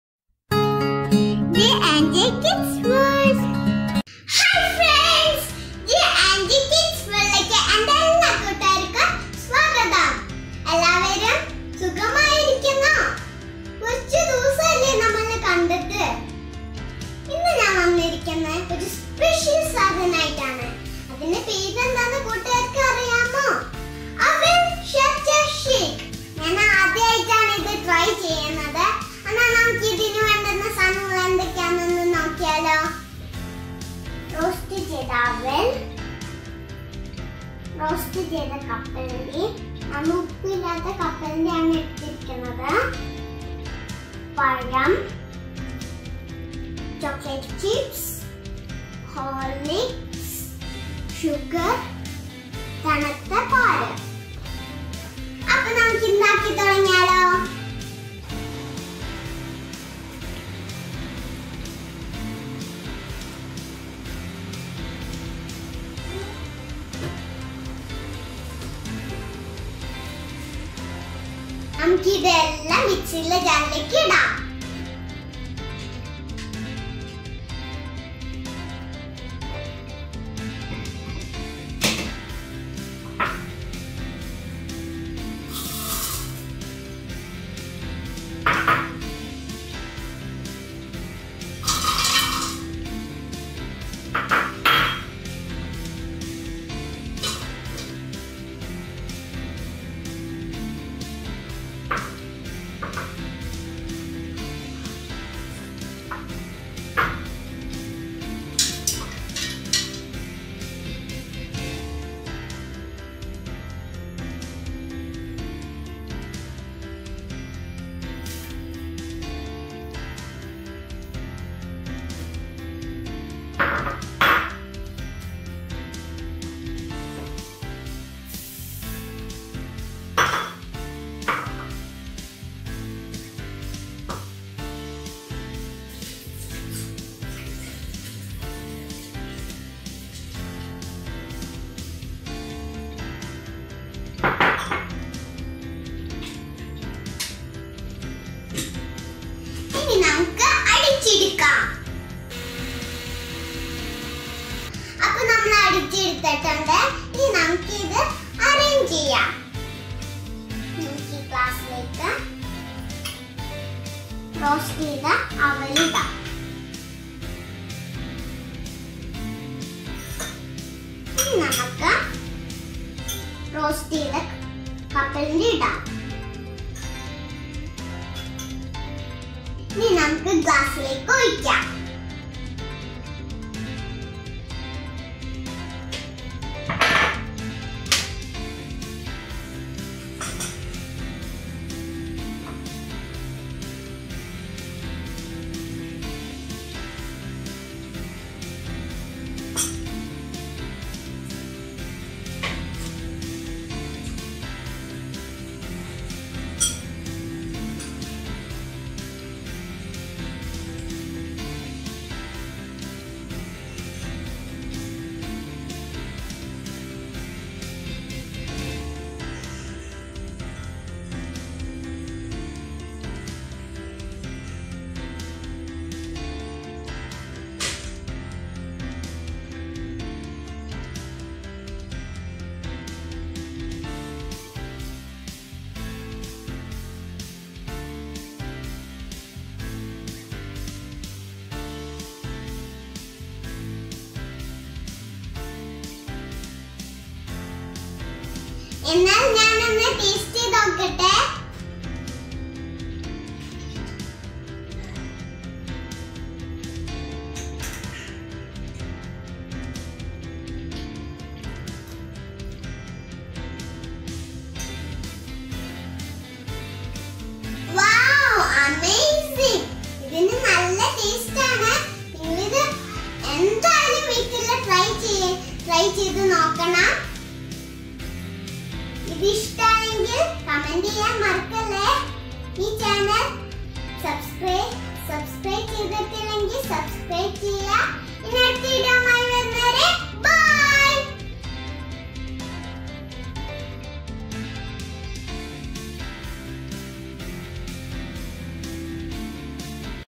Aval Sharjah Shake | അവല്‍ കൊണ്ടൊരു സൂപ്പര്‍ ഷേക്ക്‌ | Aval Milkshake | Healthy Shake |Special Shake
Aval Sharjah Shake Ingredients:

1. Roasted Aval
2. Roasted Nuts
3. 1 Banana
4. Chocolate chips
5. 2 Tablespoon Horlicks
6. 1 Teaspoon Sugar
7. 1 Cup Cold Milk